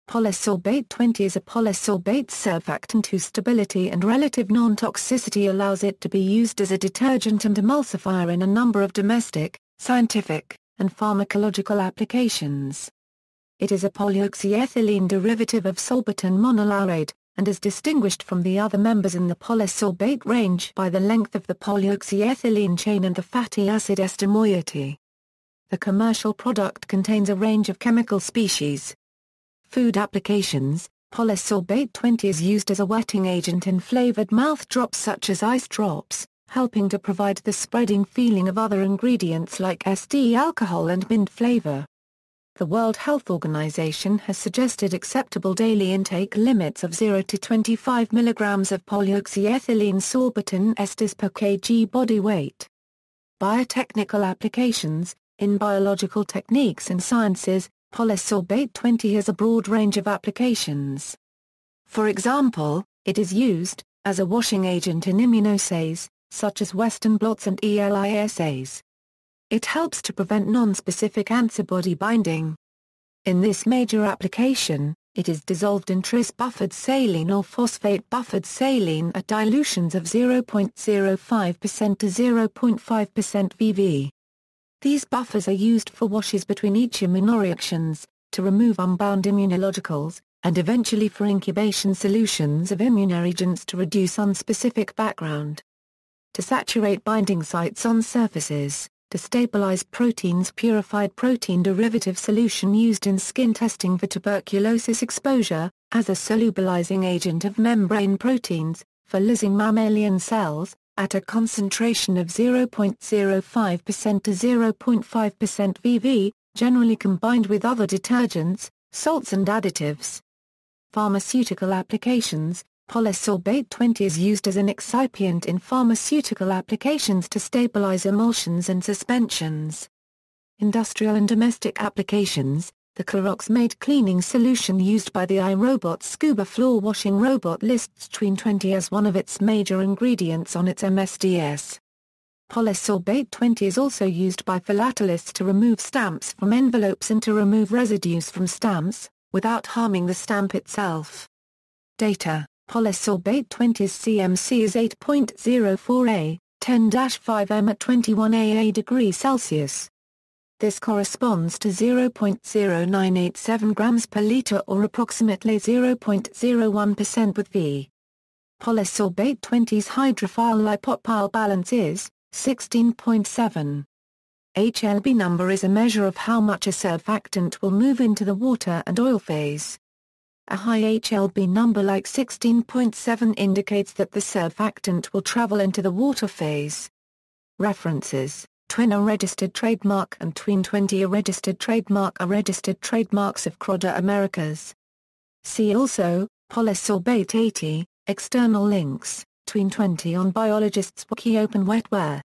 Polysorbate 20
Polysorbate 20 (common commercial brand names include Alkest TW 20 and Tween 20) is a polysorbate surfactant whose stability and relative non-toxicity allows it to be used as a detergent and emulsifier in a number of domestic, scientific, and pharmacological applications. It is a polyoxyethylene derivative of sorbitan monolaurate, and is distinguished from the other members in the polysorbate range by the length of the polyoxyethylene chain and the fatty acid ester moiety. The commercial product contains a range of chemical species.